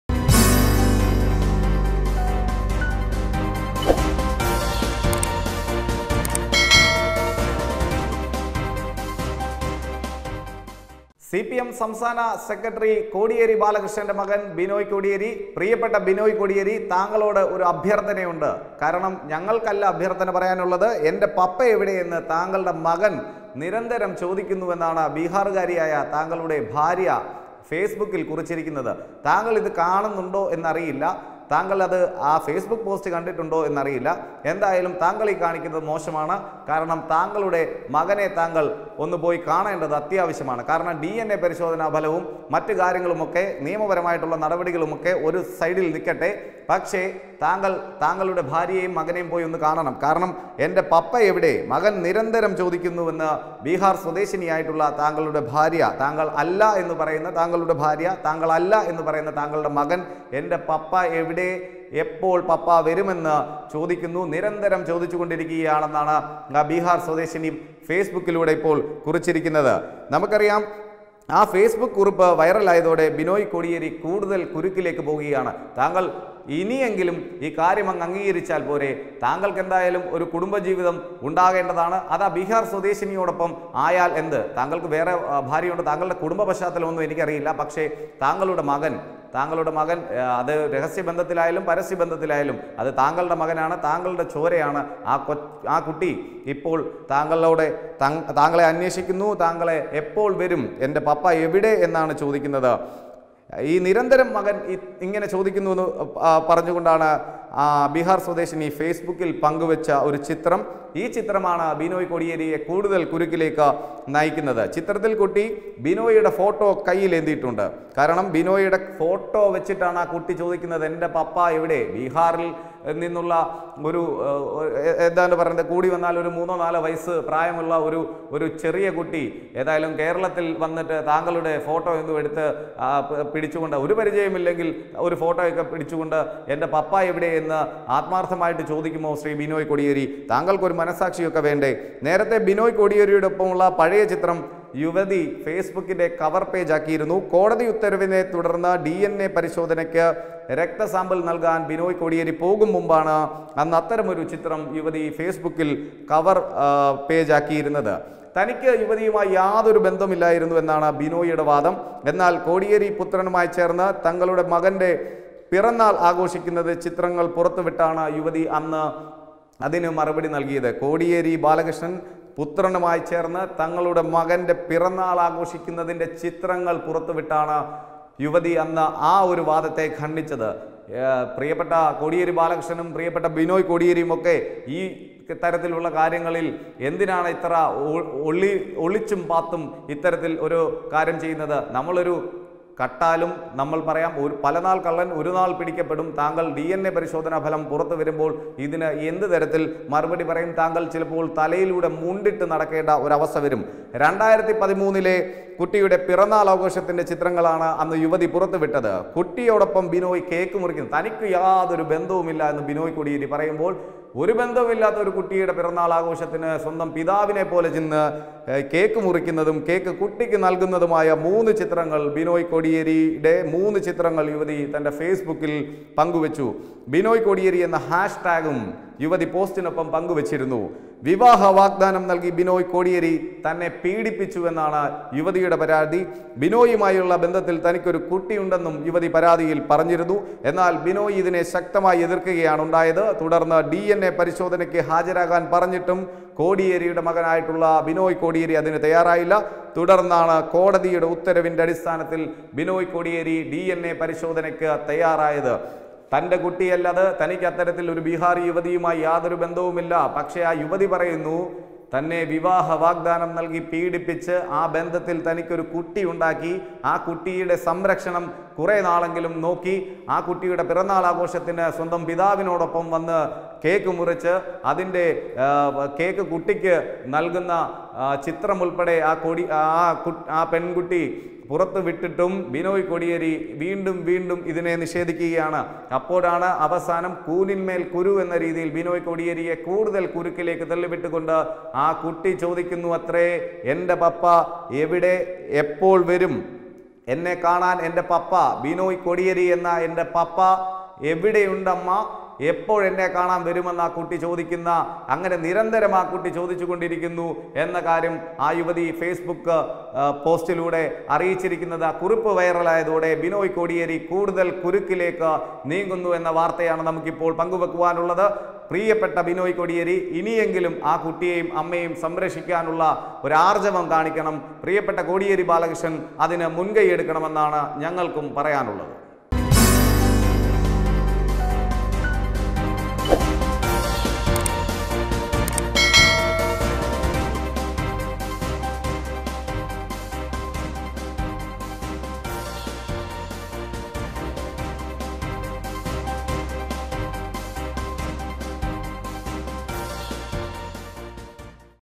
പപ്പാ...എപ്പോൾ വരും ? പെട്ടുപോയി ബിനോയ് കോടിയേരി..
ബിനോയ് കോടിയേരിയെ മകൻ ഇപ്പോഴും അന്വേഷിക്കുന്നുവെന്ന ബീഹാറി യുവതിയുടെ ഫേസ്ബുക് പോസ്റ്റ്  വൈറലായി...